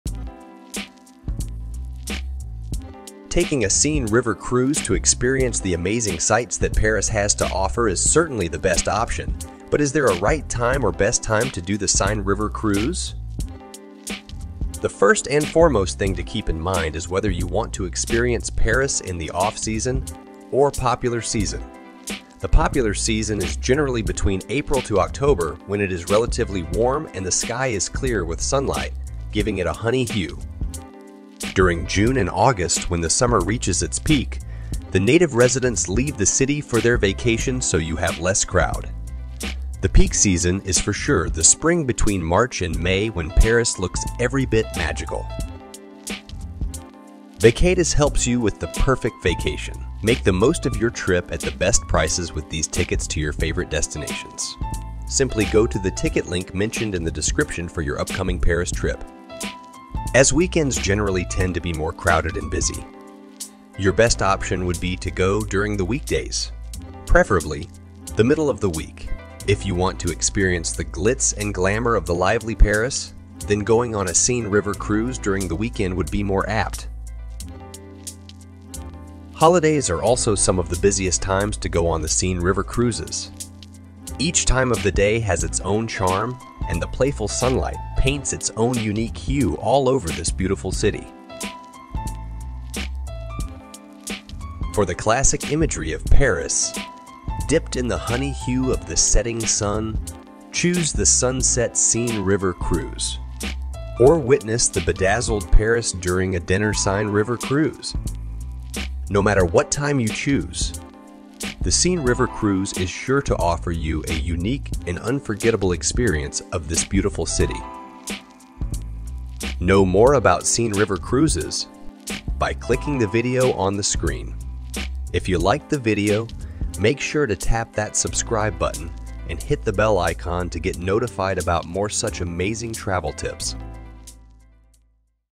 Best Time To Go On A Seine River Cruise | Paris Travel Guide
In Paris, Seine River Cruise tours are the most suitable way to get around and may include dining and wining as well. Drift through the scenic and aesthetic landmarks and atmosphere as your soak in the city.
There is never a wrong time to see Paris, but find out what is the best time to go on a Seine River cruise for you.